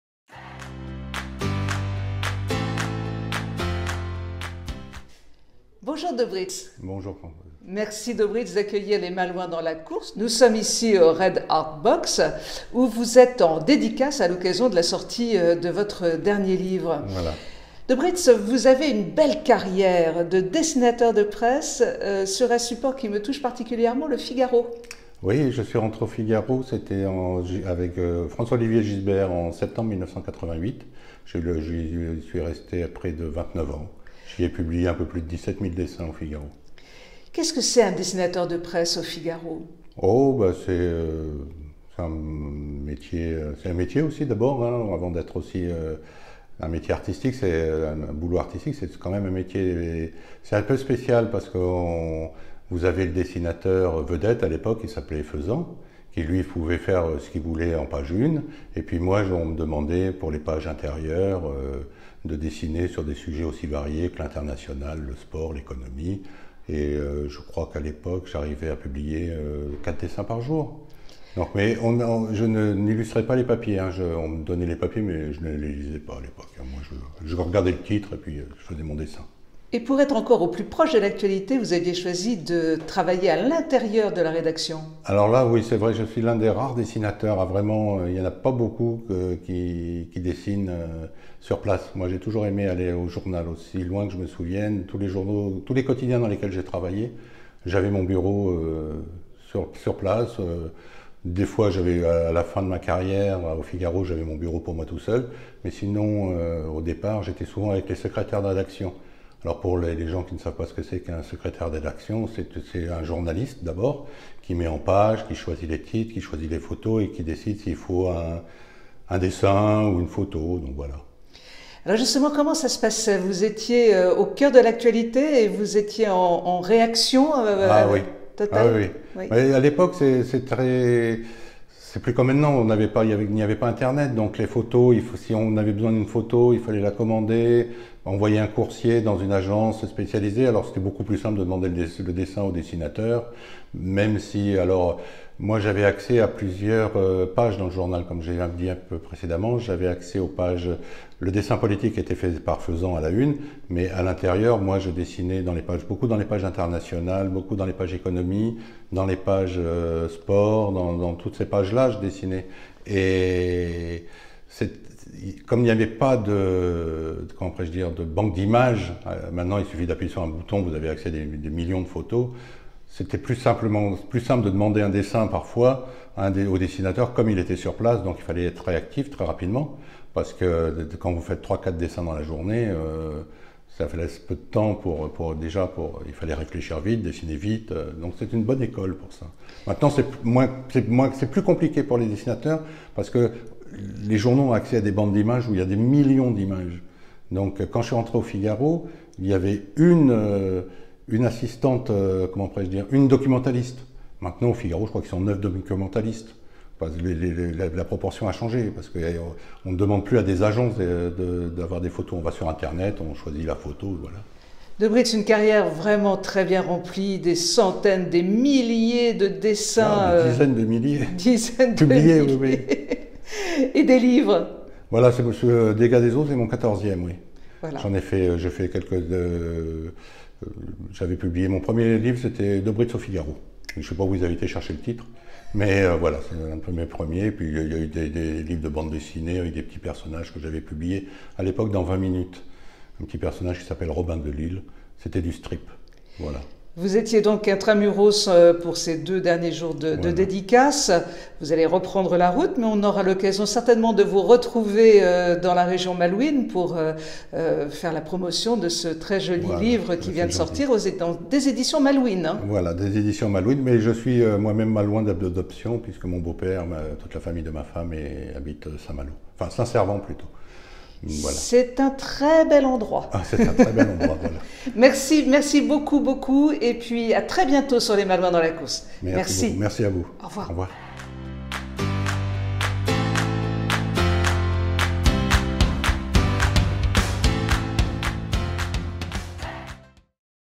DOBRITZ dessinateur de presse
Une belle carrière de dessinateur de presse de près de 30 ans au FIGARO